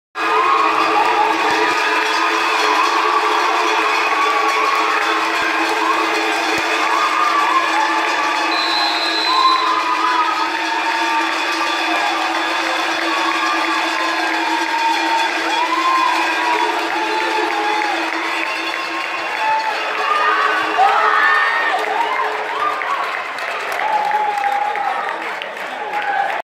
VBC Visp feiert den Aufstieg in die NLB
Der VBC Visp hat den Aufstieg in die NLB geschafft. Der Jubel ist gross, denn es wurde Geschichte geschrieben.